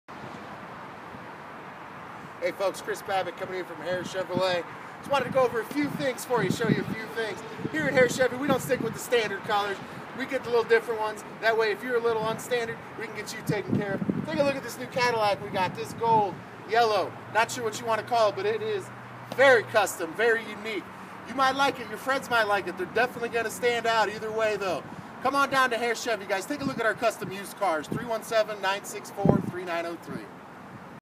Used CUSTOM GOLD Cadillac at Hare Chevrolet
We have a wide variety of used custom vehicles on our lot! Including this Gold Cadillac! Come see us today and drive off in this unique car today with little to no money down!!